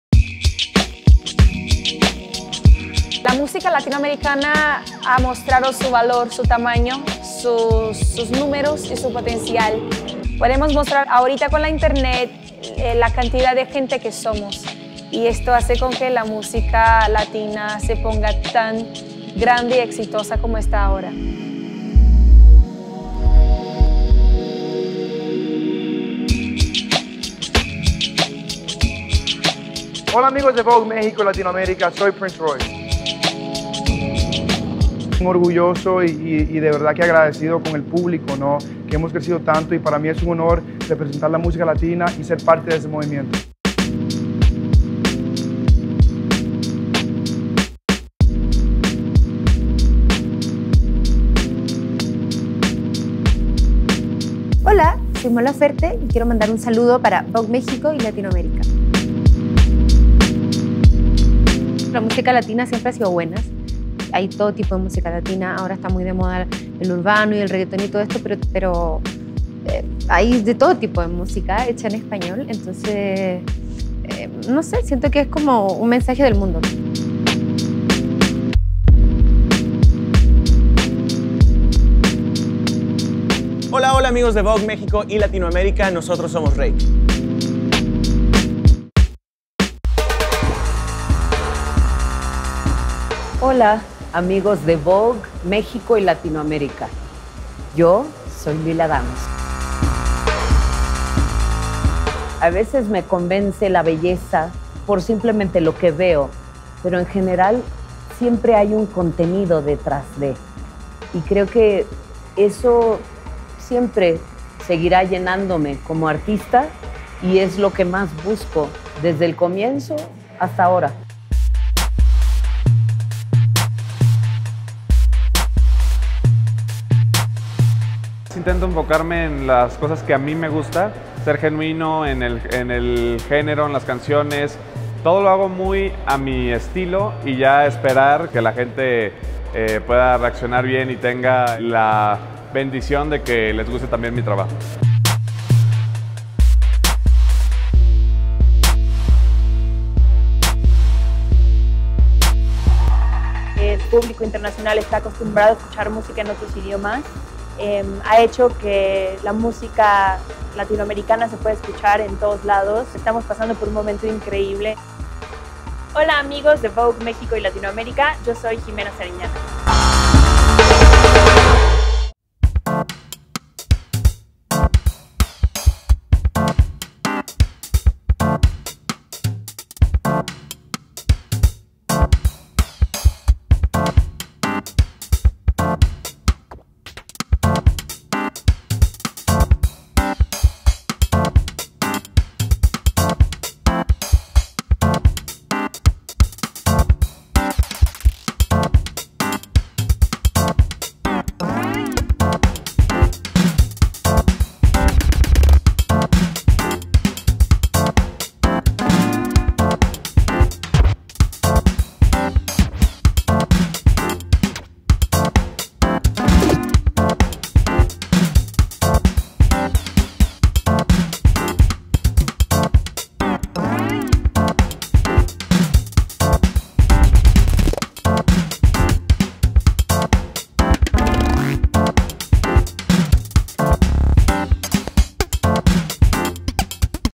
Anitta, Ximena Sariñana, Danna Paola, Reik y Luis Fonsi: la música latina está de moda
Para la edición de agosto, Vogue hace un recuento de los artistas que están subiendo el volumen de la música a nivel mundial. Mon Laferte, Reik, Prince Royce, Luis Fonsi, Ximena Sariñana, Danna Paola,  Lila Downs, Anitta, Alex Fernández y Laura Tobón.